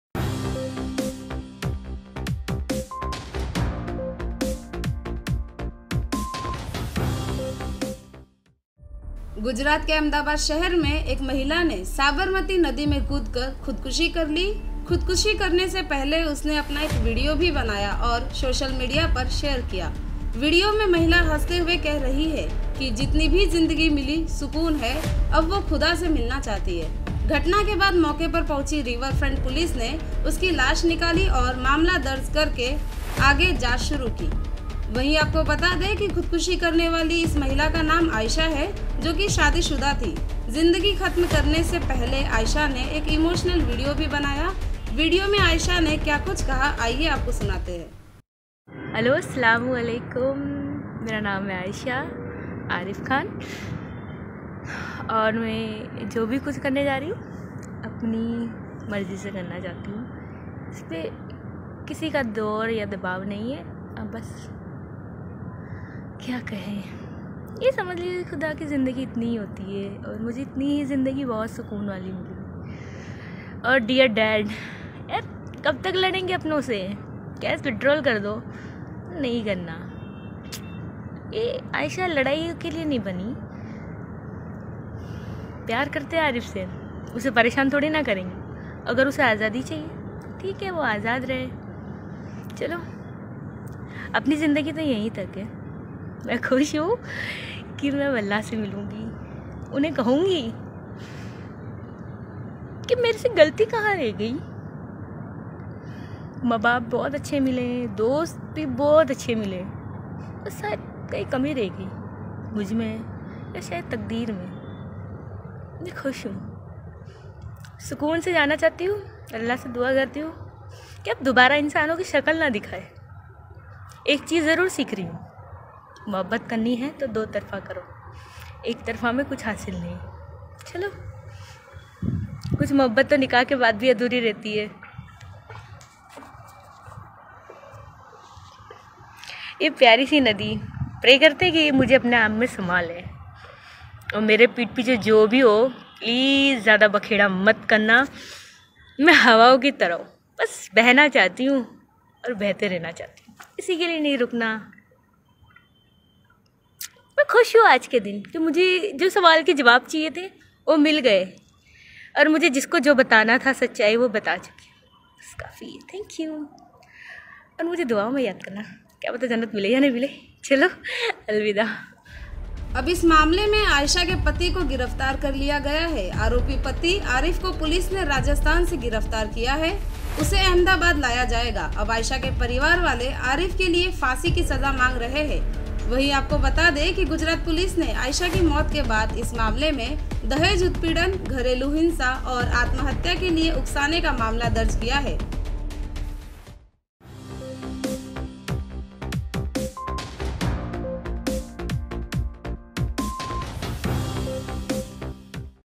आयशा ने खुदकुशी करने से पहले बनाया आखरी वीडियो ।Ayesha Suicide Case
आयशा ने खुदकुशी करने से पहले बनाया अंतिम वीडियो ।।DRV NEWS
सुसाइड के पहले आयशा ने बनाया वीडियो
वीडियो बनाकर सोशल मीडिया पर किया शेयर
अहमदाबाद की आयशा ने की खुदकुशी
Ayesha made the last video before committing suicide.
Ayesha made video before suicide
Ayesha of Ahmedabad commits suicide